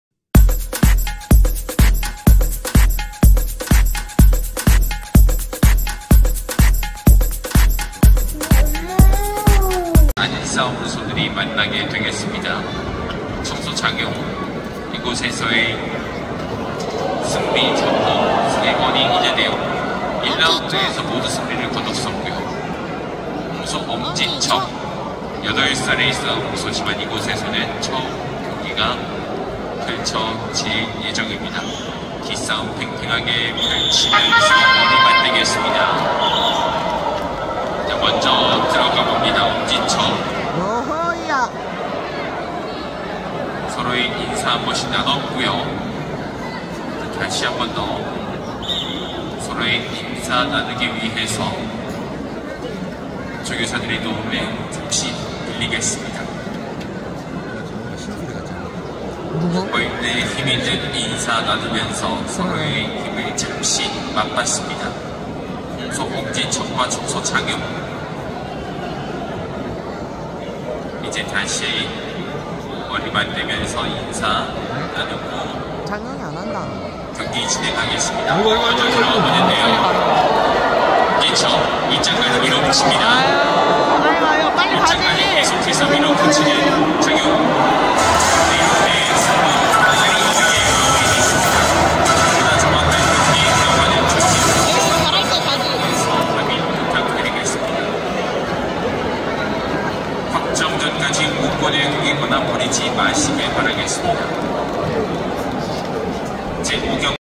소싸움 👍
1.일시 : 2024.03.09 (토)

2.장소 : 청도 소싸움 경기장

3.출전소
 - 홍소 : 엄지척
 - 청소 : 장용

4.결과 : 1라운드 엄지척 승